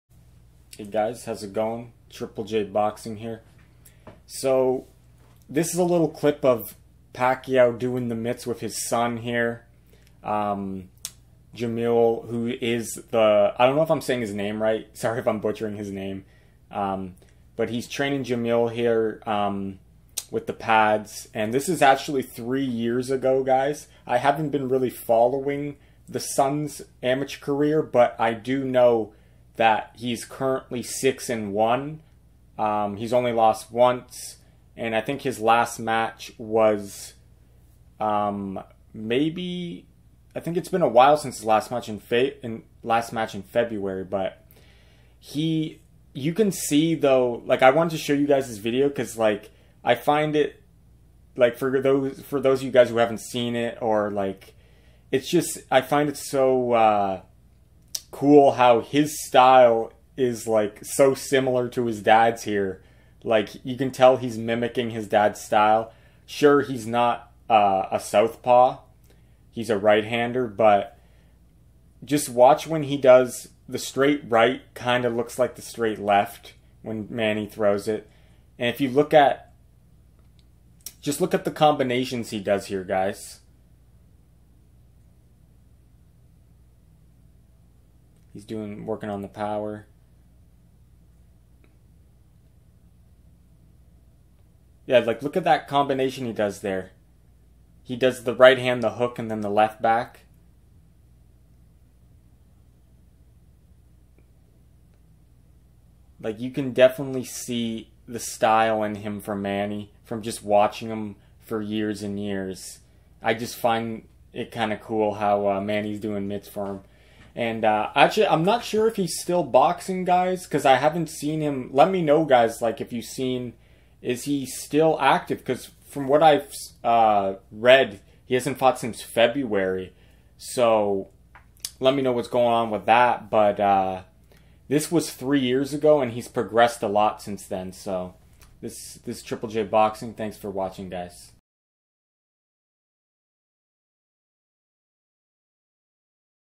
Manny Pacquiao doing the mitts with his son Jimuel
JJJBoxing
Credit: MannyPacquiao